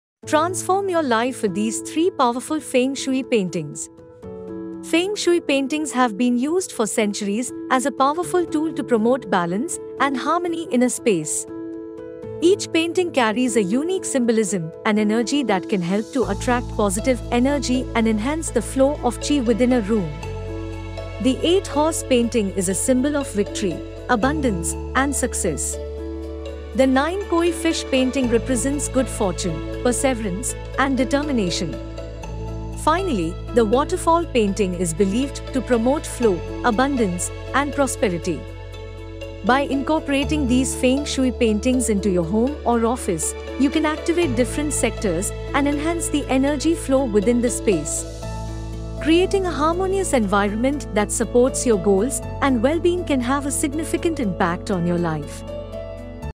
Transform Your Home with the Best Feng Shui Paintings for Wealth | Top 3 Feng Shui Paintings
In this video, we showcase the three best feng shui paintings to help you create a harmonious and prosperous living space. Our expert feng shui practitioners have handpicked these paintings based on their ability to attract positive energy and promote balance and well-being.

You'll learn about the principles of feng shui and how these paintings can help you harness the power of qi or life force energy. We explore the symbolism behind each painting and how they can enhance specific areas of your home, such as your wealth, health, and relationships.

Discover how these feng shui paintings can help you achieve your goals, whether it's to increase your wealth, improve your health, or strengthen your relationships. With our expert guidance, you'll be able to select and incorporate the perfect feng shui paintings into your living space, creating a sanctuary that supports your well-being and success.

Transform your home and life with the power of feng shui and the best feng shui paintings available. Watch this video and discover how these paintings can help you attract abundance, harmony, and joy.